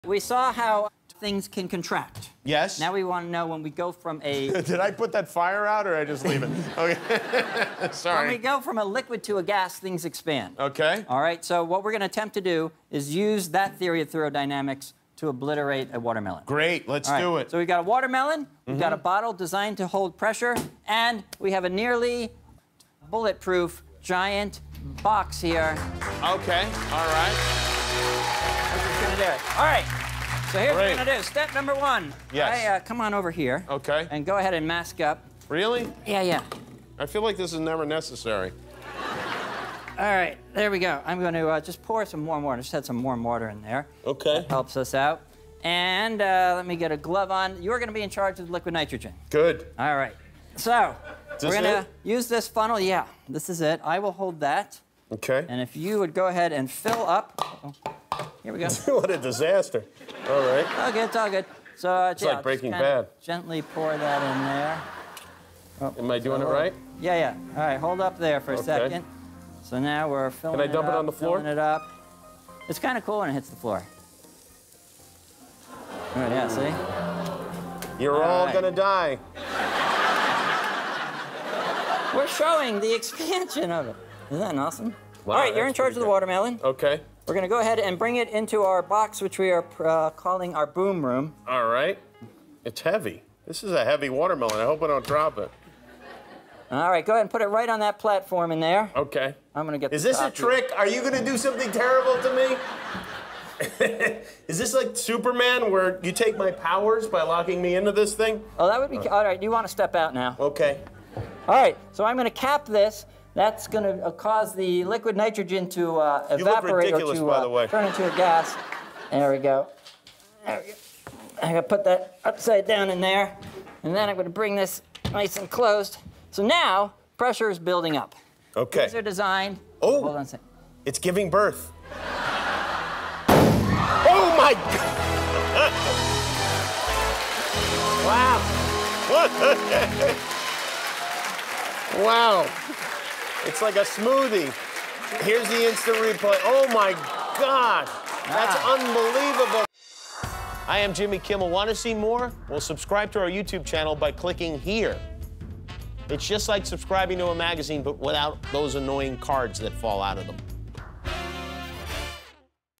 Science Bob Blows Up a Watermelon
Bob uses science to explode a watermelon.